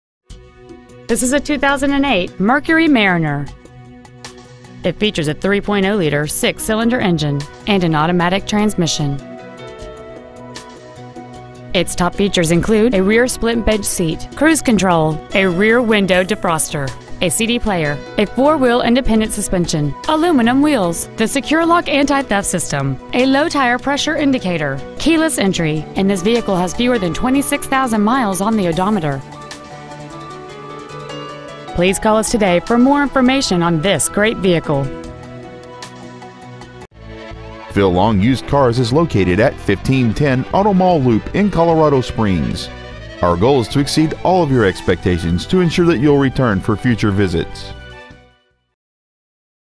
2008 Mercury Mariner Denver CO
Year: 2008
 Make: Mercury
 Model: Mariner
 Engine: 3.0L SFI Duratec V6 engine
 Trans.: Automatic
 Exterior: White
 Miles: 25,787
 Interior: Gray

 Preowned 2008 Mercury Mariner Colorado Springs CO